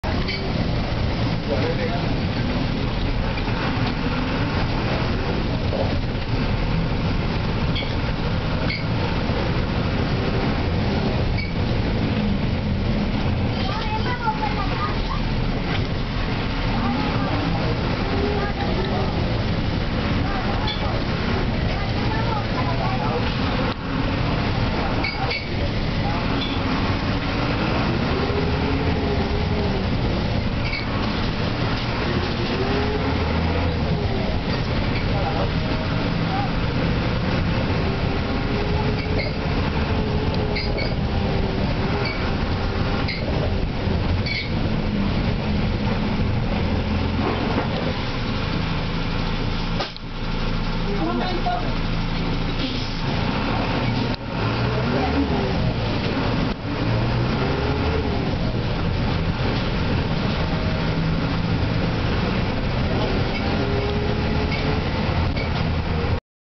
Por las trochas de Bogotá
Caminos de herradura, polvorientas demostraciones de la incompetencia de los dirigentes de una urbe de más de 8 millones de personas en pleno siglo XXI.